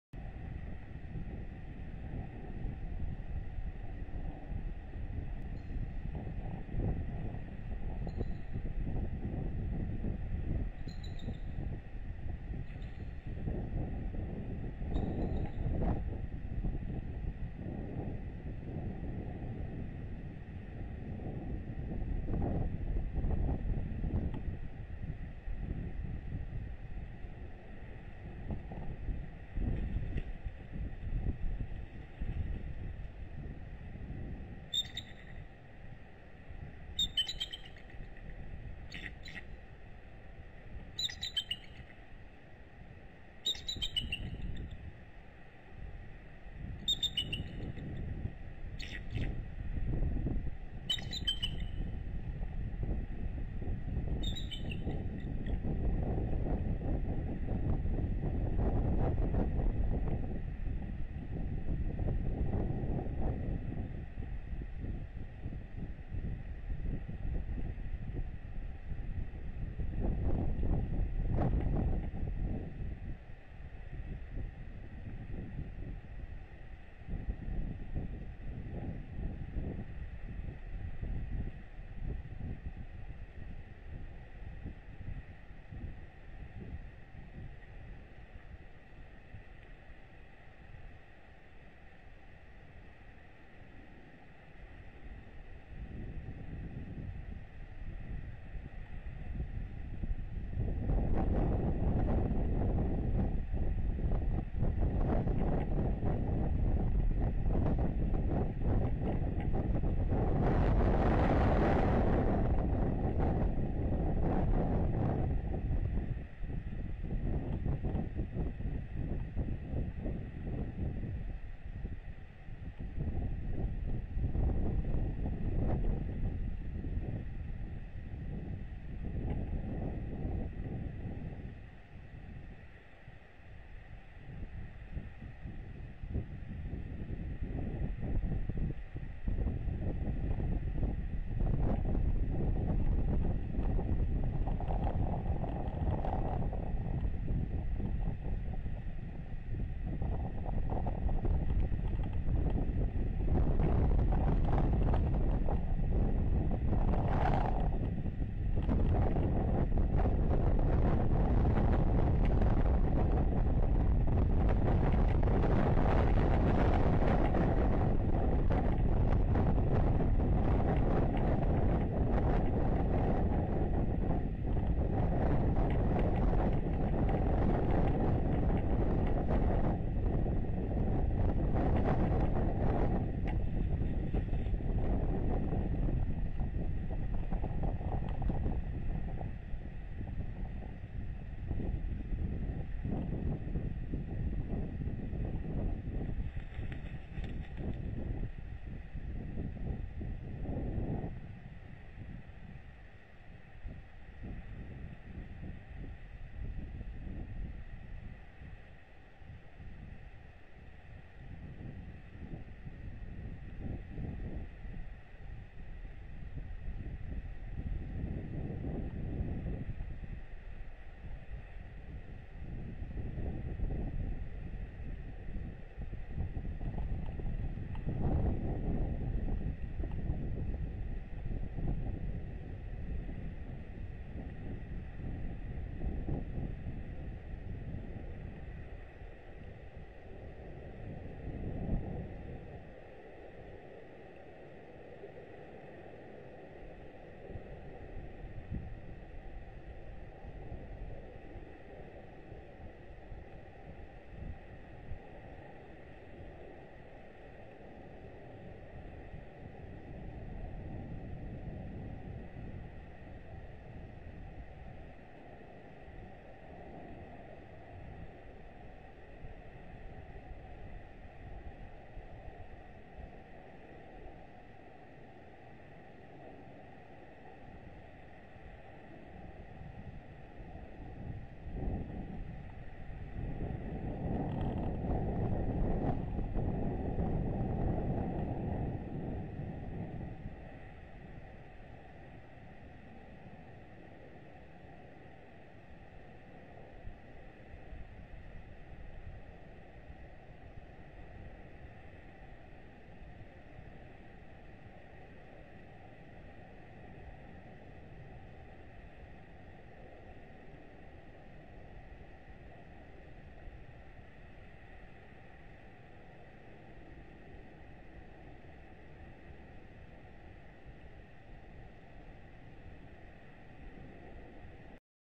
DHEC Obey Flies In To Take Over Incubating River Flies Off 2018 03 01
Obey flies in 21:22, River flies off 21:25